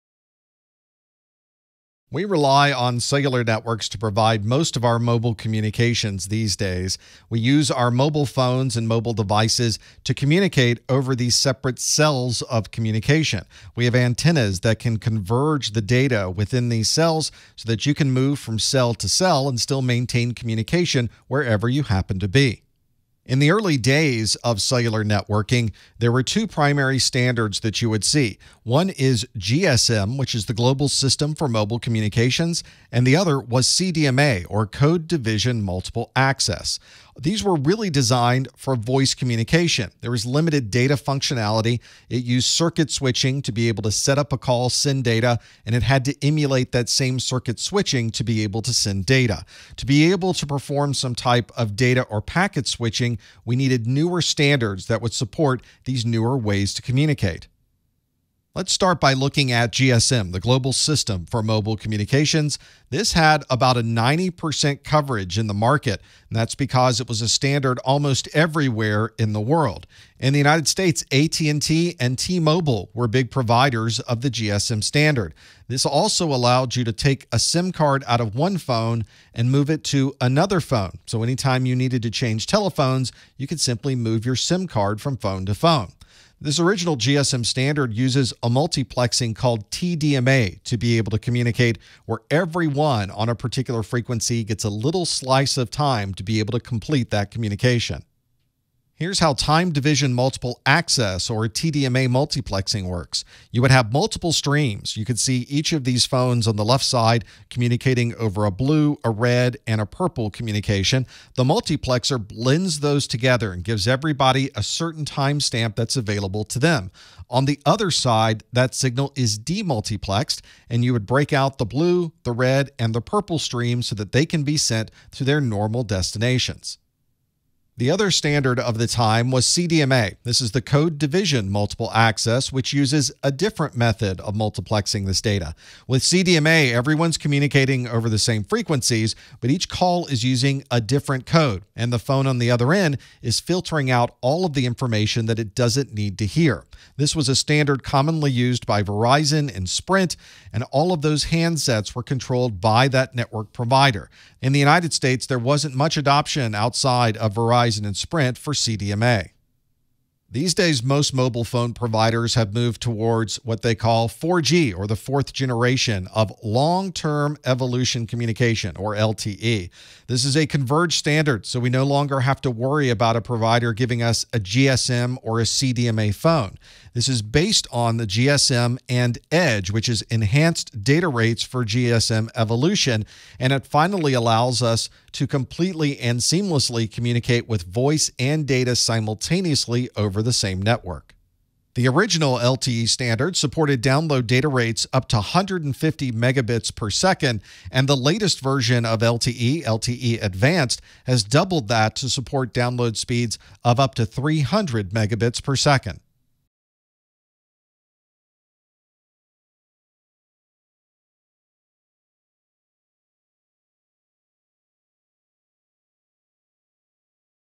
Cellular Network Standards - CompTIA Network+ N10-007 - 1.6
Cellular networks are complex technologies that have been made easy enough for everyone to use. In this video, you’ll learn about GSM, CDMA and LTE cellular technologies.